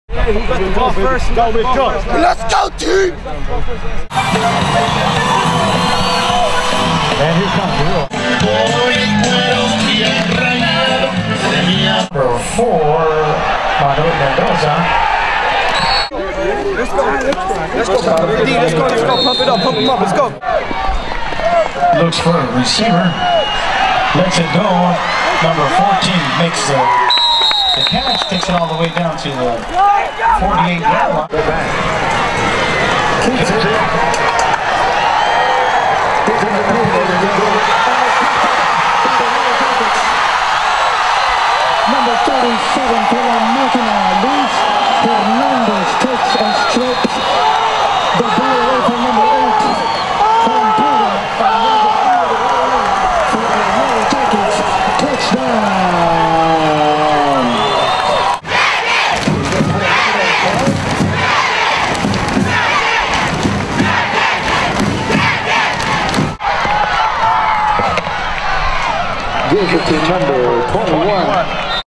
La Maquina stay undefeated at Beny Layton Sr. Stadium
The Yellow Jackets hosted the Rio Hondo Bobcats and caged them fast entering half time with a large lead and they didn't look back as they eased their way into a 34-14 rampage.